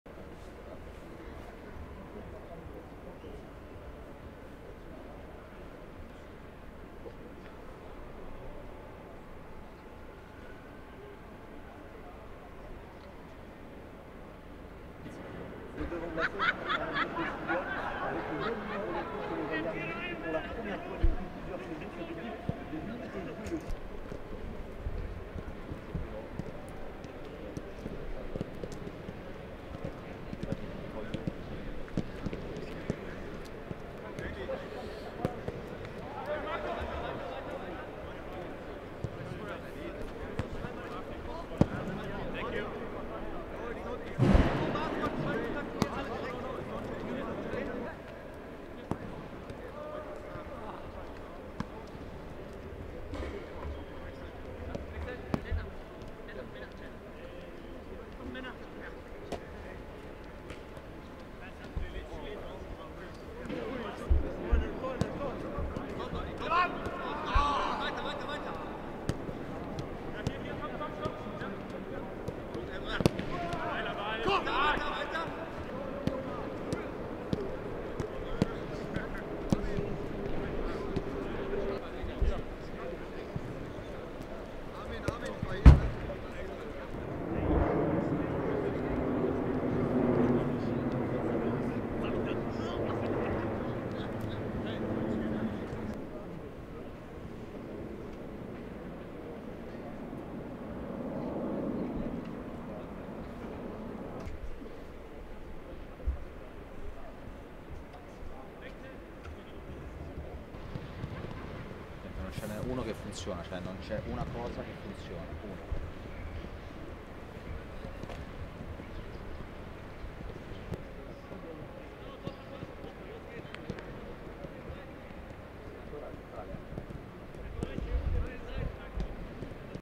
Dortmund train at Stamford Bridge ahead of UCL last 16 tie with Chelsea｜Champions League
Borussia Dortmund trained at London's Stamford Bridge on Monday as the Bundesliga side look to reach the last eight of the UEFA Champions League when they play Chelsea on Tuesday.